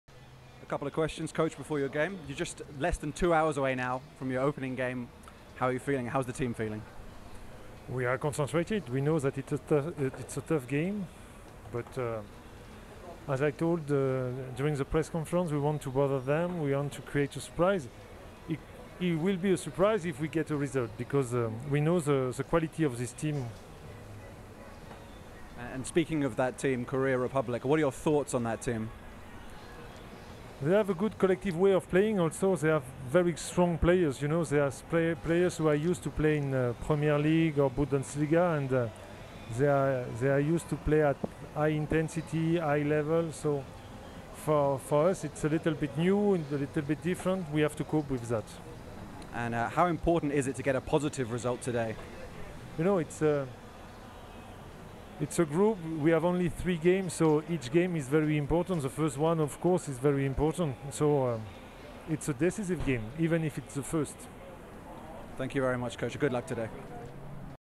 Flash Interview: Match 2: Paul Le Guen (Oman Coach)
Oman coach Paul Le Guen speaks before the opening AC2015 game with Korea Republic.